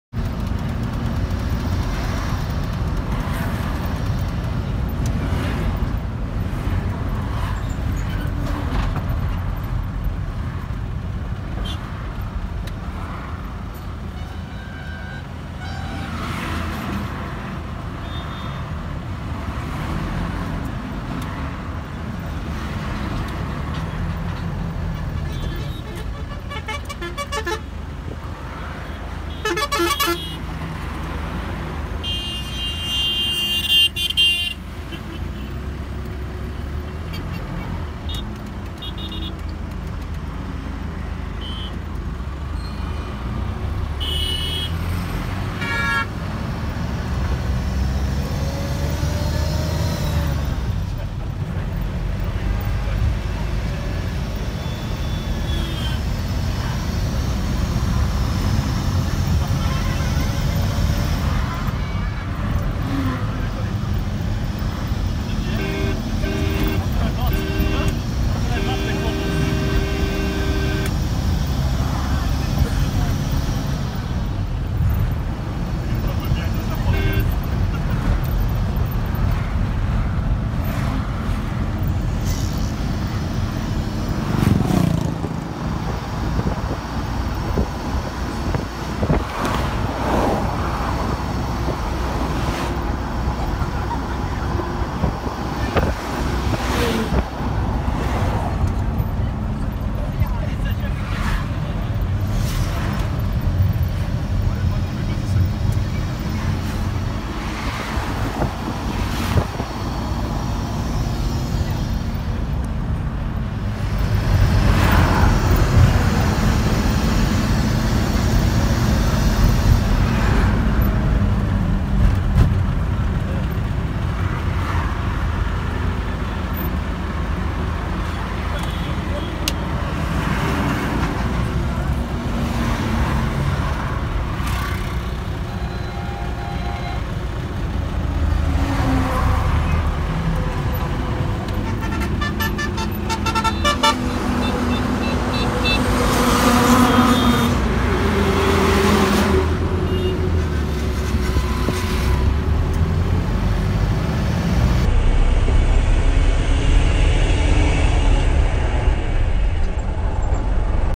Ahmedabad Surat Traffic Jam
To avoid traffic jam in India, drive on the opposite side of the road.
Good driving bhai.